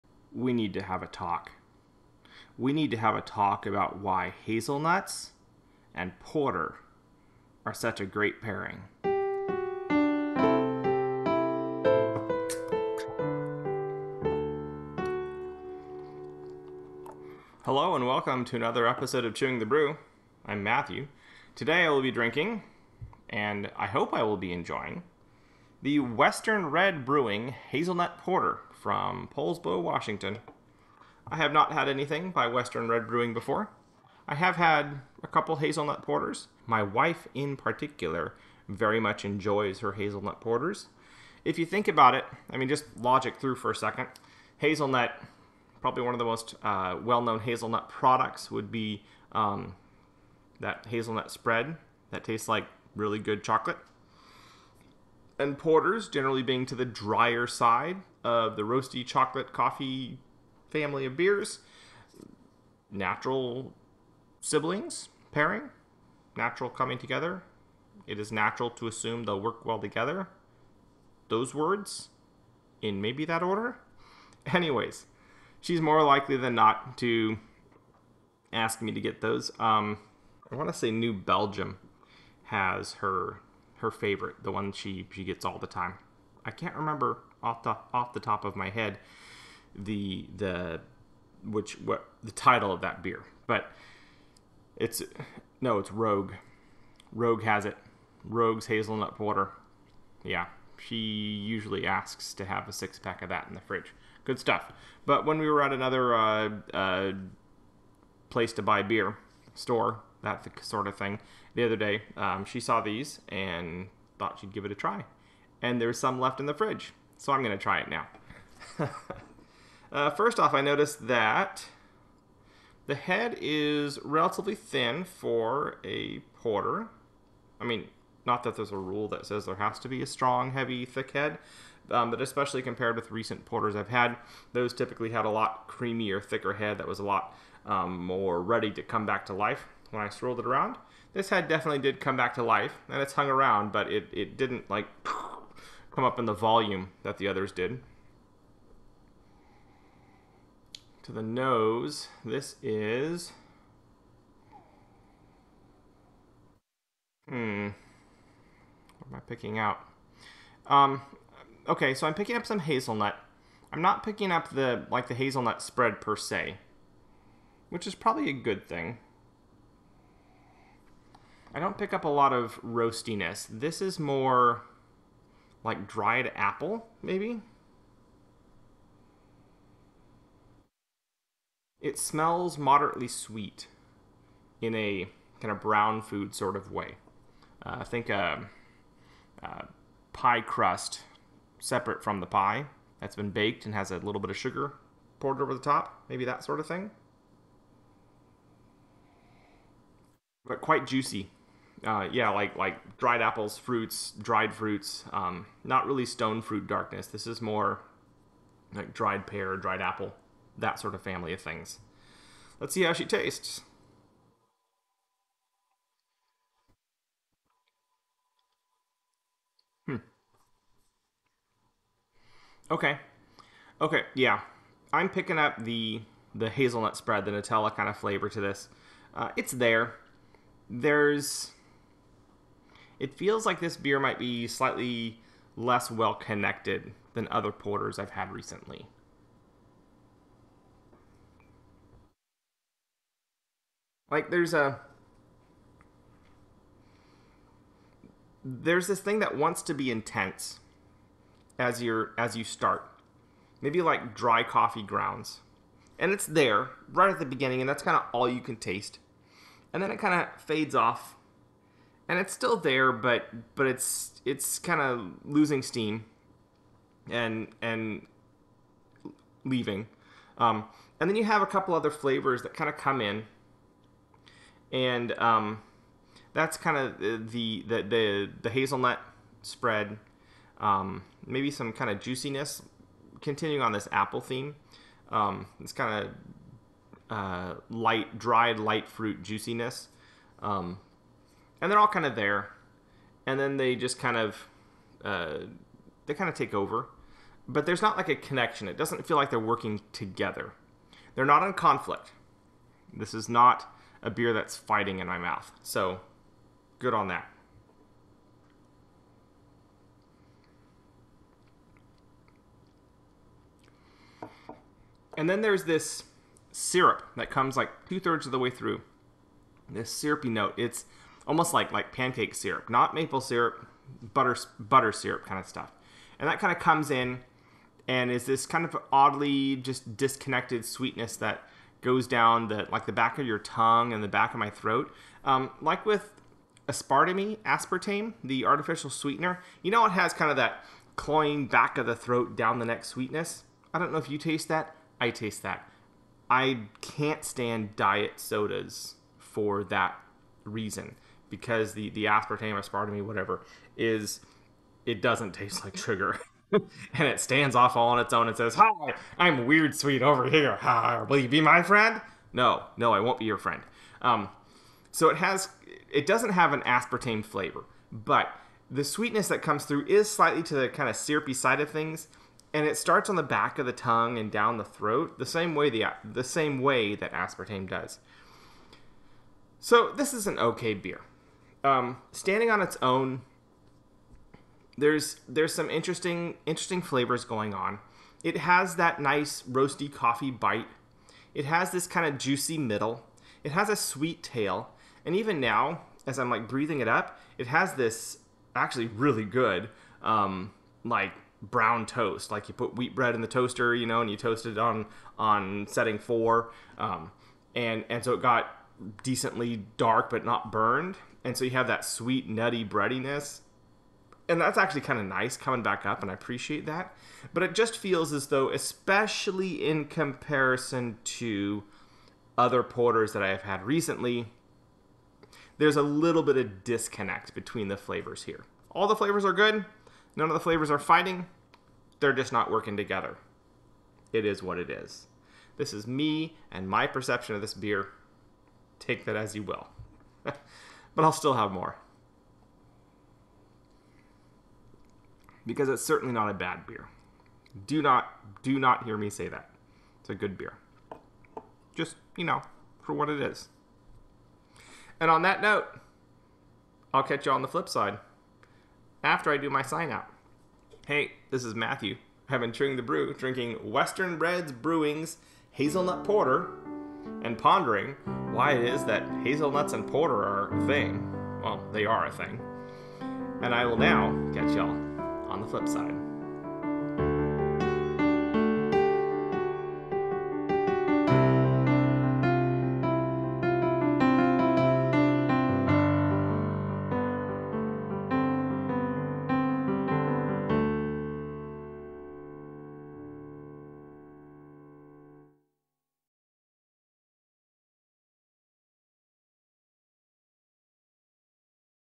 Western Red Brewing Hazelnut Porter #103
My first Western Red Brewing beer is this hazelnut porter. It's an entirely adequate and enjoyable beer, but not so smooth or sweet as other hazelnut porters I've had.